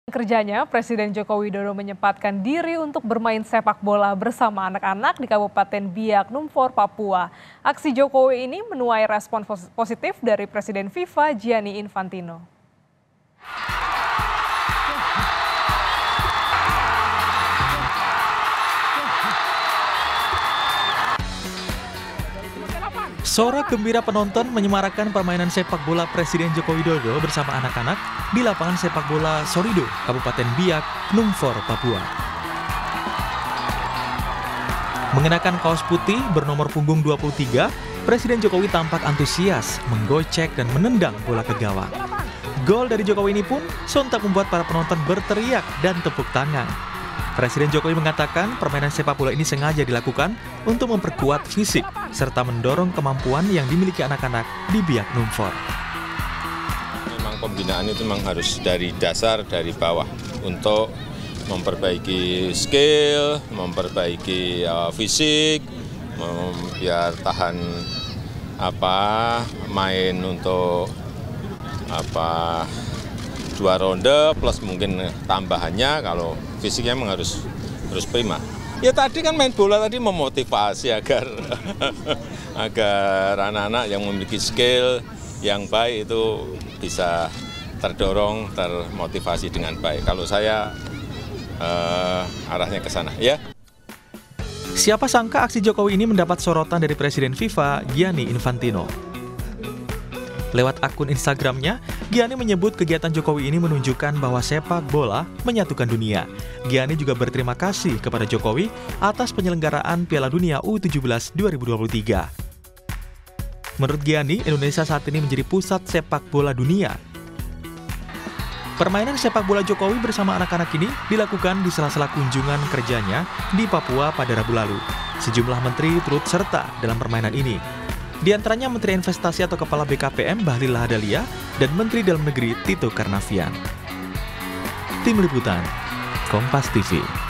Aksi Jokowi Main Bola Bareng Anak Papua Disorot Presiden FIFA Gianni Infantino
KOMPAS.TV - Di sela kunjungan kerjanya, Presiden Joko Widodo menyempatkan diri untuk bermain sepak bola bersama anak-anak di Kabupaten Biak Numfor, Papua.

Aksi Jokowi ini menuai respon positif dari Presiden FIFA Gianni Infantino.

Sorak gembira penonton menyemarakkan permainan sepak bola Presiden Joko Widodo bersama anak-anak di lapangan Sepak Bola Sorido, Kabupaten Biak Numfor Papua.

Mengenakan kaus putih bernomor punggung 23 Presiden Jokowi tampak antusias menggocek dan menendang bola ke gawang.

Siapa sangka aksi Jokowi ini mendapat sorotan dari Presiden FIFA Gianni Infantino.

Lewat akun Instagram-nya, Gianni menyebut kegiatan Jokowi ini menunjukkan bahwa sepak bola menyatukan dunia.

Gianni juga berterima kasih kepada Jokowi atas penyelenggaraan Piala Dunia U-17 2023.

Baca Juga Thailand Ajak Indonesia Jadi Tuan Rumah Kejuaraan Dunia Bola Voli Wanita FIVB 2025 di